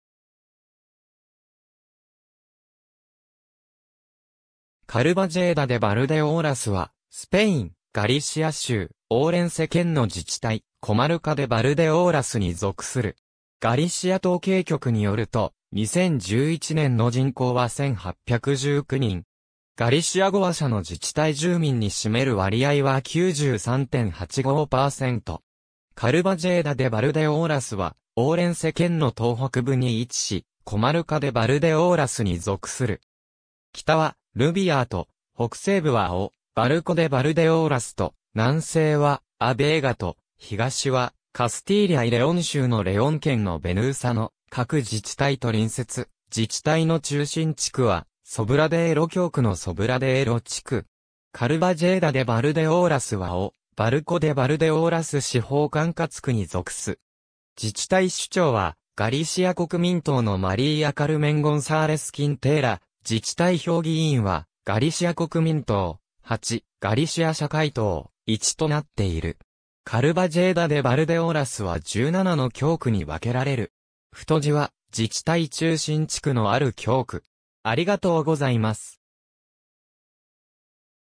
カルバジェーダ・デ・バルデオーラス
カルバジェーダ・デ・バルデオーラス（Carballeda de Valdeorras）はスペイン、ガリシア州、オウレンセ県の自治体、コマルカ・デ・バルデオーラスに属する。
ガリシア統計局によると、2011年の人口は1,819人（2010年：1,866人、2009年：1,870人、2004年：2,080人、2003年：2,155人）。
カルバジェーダ・デ・バルデオーラスはオウレンセ県の東北部に位置し、コマルカ・デ・バルデオーラスに属する。
北はルビアーと、北西部はオ・バルコ・デ・バルデオーラスと、南西はア・ベイガと、東はカスティーリャ・イ・レオン州のレオン県のベヌーサの各自治体と隣接、自治体の中心地区はソブラデーロ教区のソブラデーロ地区。
カルバジェーダ・デ・バルデオーラスはオ・バルコ・デ・バルデオーラス司法管轄区に属す。
自治体首長はガリシア国民党（PPdeG）のマリーア・カルメン・ゴンサーレス・キンテーラ（María Carmen González Quintela）、自治体評議員はガリシア国民党：8、ガリシア社会党（PSdeG-PSOE）：1となっている（2011年5月22日の自治体選挙結果、得票順）。
カルバジェーダ・デ・バルデオーラスは17の教区に分けられる。